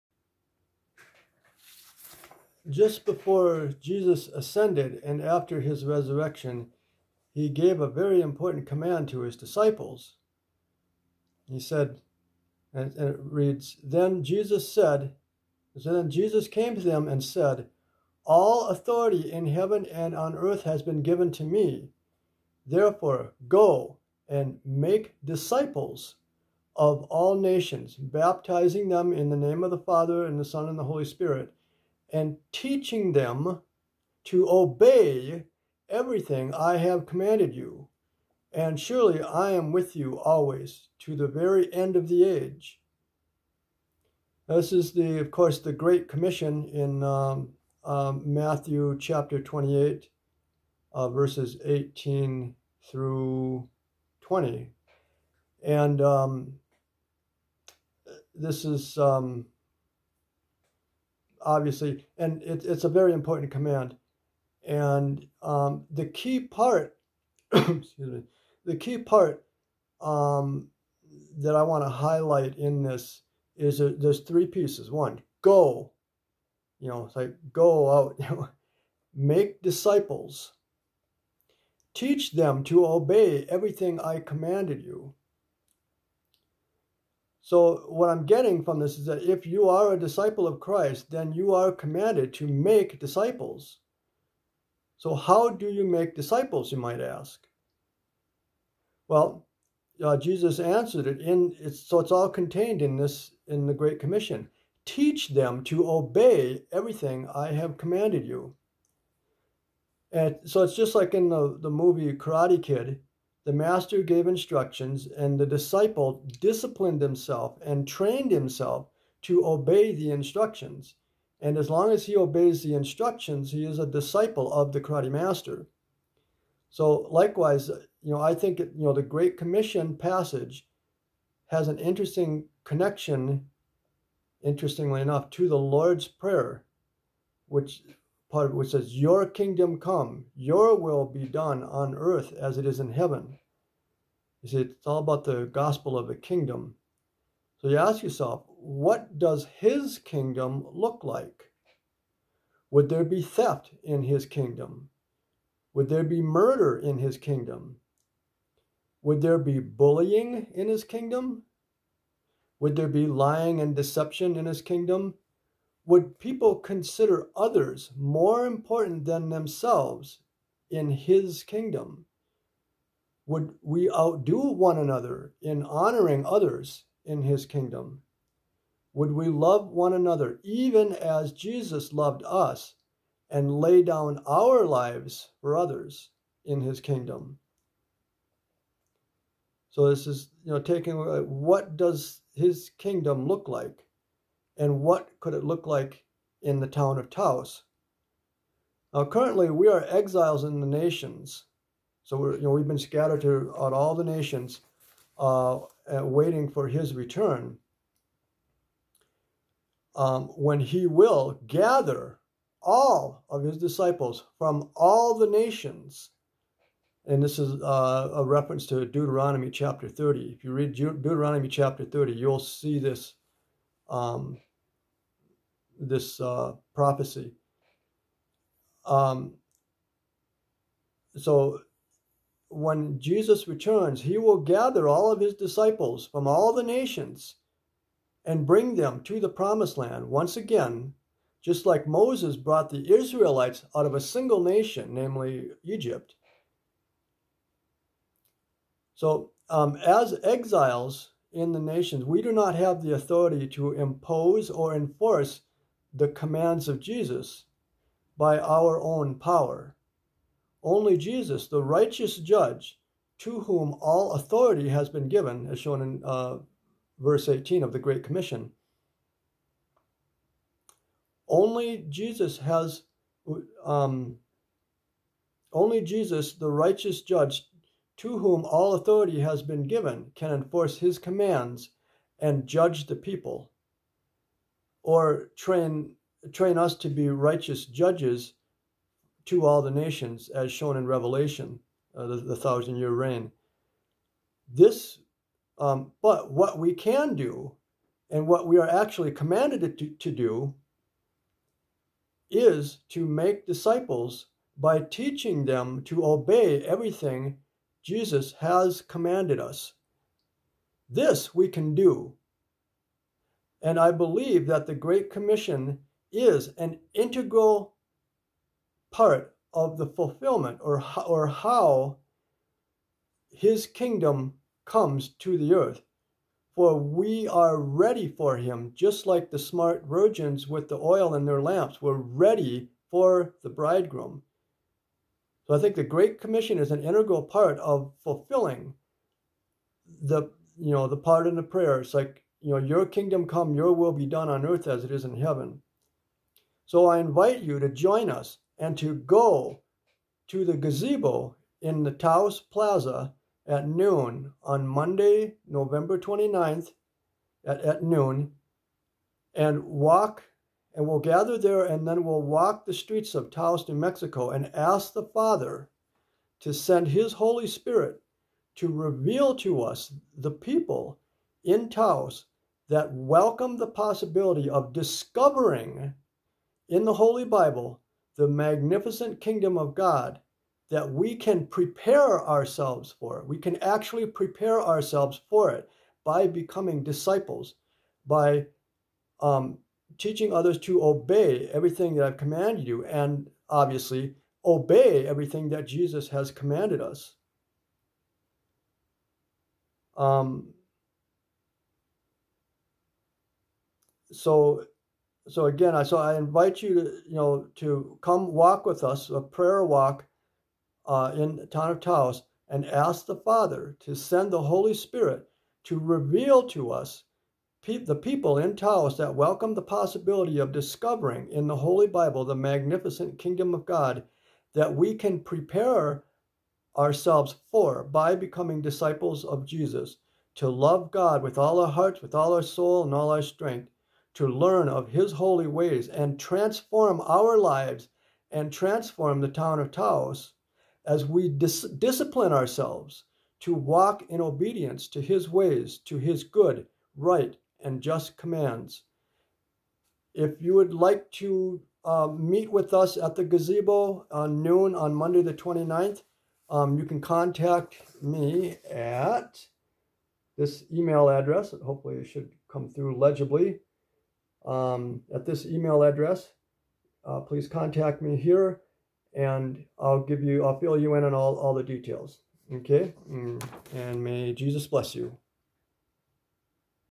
Prayer Walk-a-Thon for Taos!
Prayer Walk-a-Thon for the Town of Taos.
The prayer walk starts at Noon, Monday November 29, 2021 at the gazebo in Taos Plaza. We will walk along the main street of the town of Taos and some side streets.

As we walk and pray imagine what would Taos look like even if just one of Jesus' commands was obeyed? What if there was no theft in Taos? What would that look like? Would Taos be transformed by this one thing? What would it look like if there was no more bullying in Taos? Would Taos be transformed by this one thing? Or how about adultery? Would relationships be transformed by this one thing? What if people considered others more important than themselves? Would that one thing transform Taos?
Consider Philippians 2:
"3Do nothing out of selfish ambition or empty pride, but in humility consider others more important than yourselves."
Such an instruction, though not directly stated by Jesus, is implied in Jesus's command to love one another even to laying down your life for the sake of others. What kind of impact would that have on the town of Taos? Can you imagine?!

So if you feel called, please join us for the prayer walk that starts at the Taos plaza gazebo at Noon, Mon, November 29, 2021.